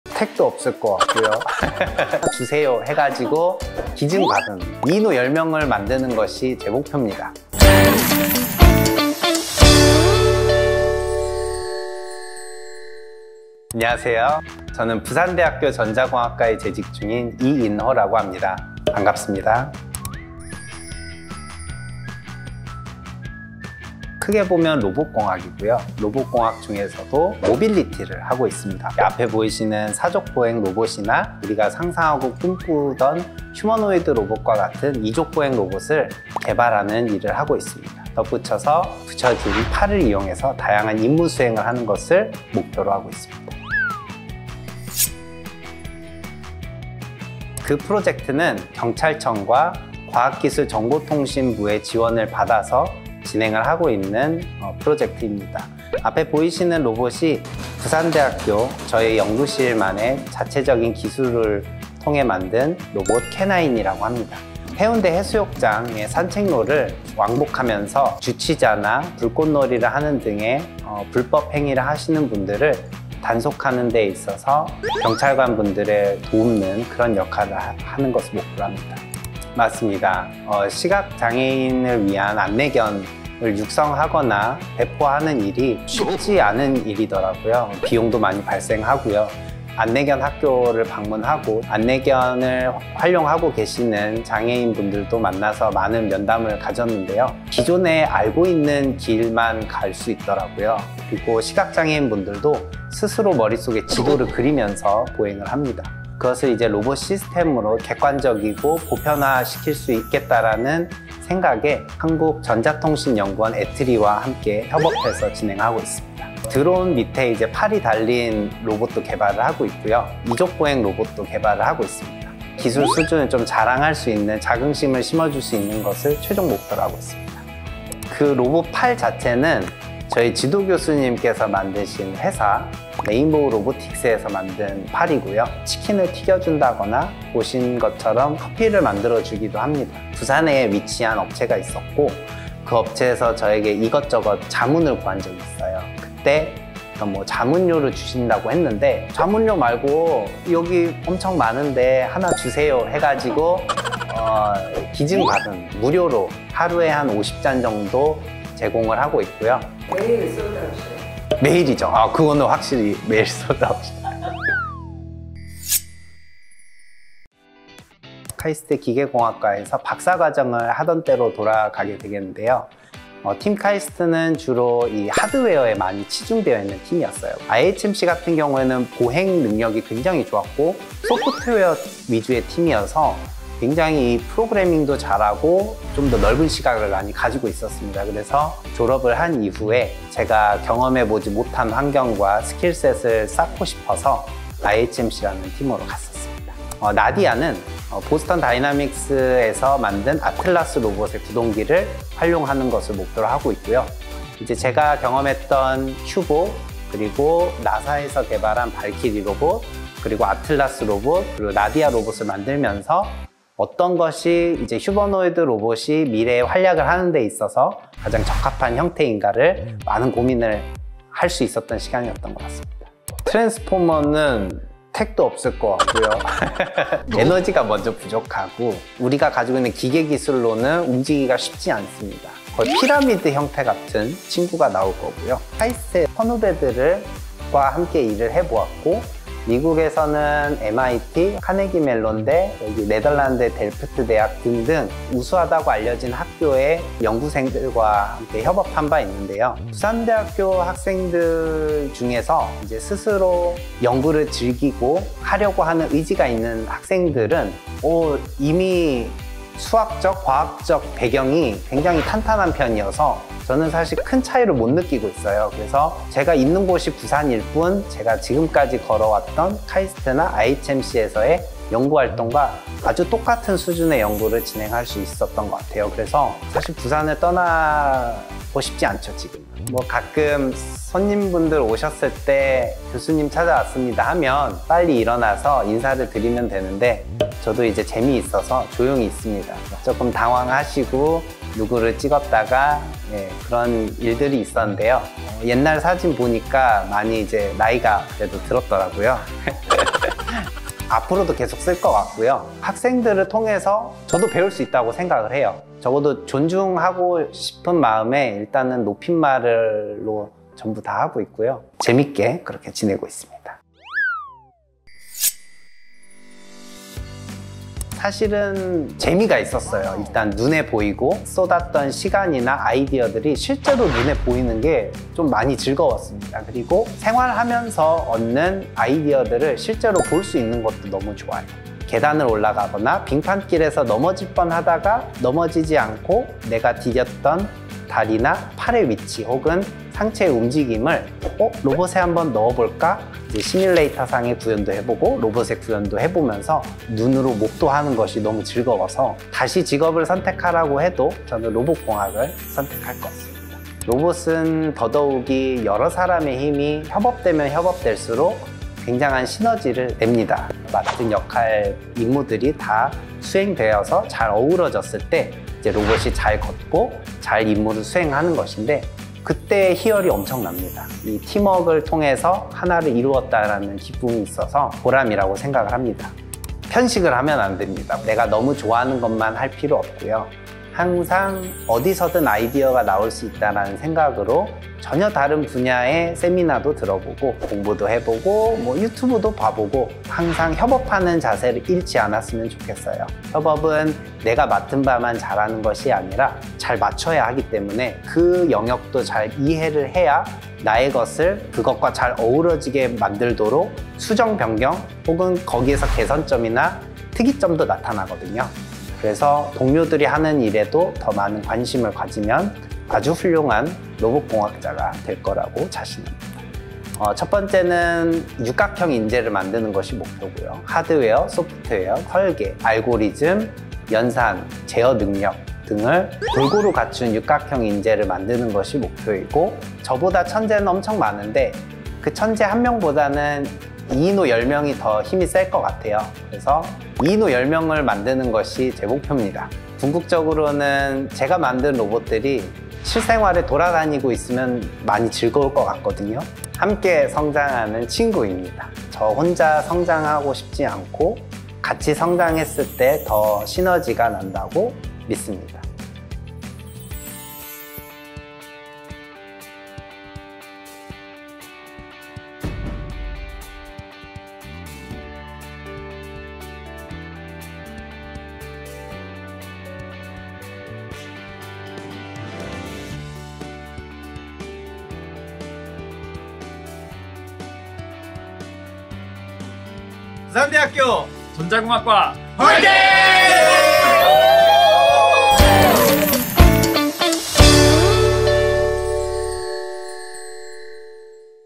[피어나다] 부산대학교 공과대학 전자공학과 「이인호 교수편」
젊은 로봇공학자 이인호 교수는
휴머노이드 로봇의 메카인
KAIST와 미국 IHMC를 거쳐
부산대학교 CAMEL연구실(Computer And Machine
 Engaged Lab.)을 운영하며,
부산대 연구생들과 4족 보행 로봇을 개발 중인데요.

한 명의 천재보다 열 명의 이인호가
더 많은 시너지를 발휘하는
휴머노이드 로봇 개발을 통해
부산대학교의 로봇융합 연구 우수성을
널리 알리고 싶다는데요.

앞으로 ‘메이드인 부산대학교’의 로봇들이
우리의 일상과 함께 할 그날이
멀지 않은 듯싶네요!!

★ 이인호 교수
- 부산대학교 공과대학 전자공학과 교수
- 미국 IHMC(인간-기계인지연구소) 박사후 연구원
- KAIST 휴머노이드 연구실 ‘휴보랩’ 박사후 연구원
- KAIST 기계공학과 학사, 석박사
- 세계 휴머노이드 로봇 국제컨퍼런스 최우수 논문상    (2019)

★ ‘피어나다’ 프로그램 소개
부산대학교(PNU) 구성원의
(나)다운 다양한 스토리텔링을 통해
개인·학교·지역의 소통, 화합, 발전하는
중의적 (피어남)을 의미

ⓒ 부산대학교 By. 홍보실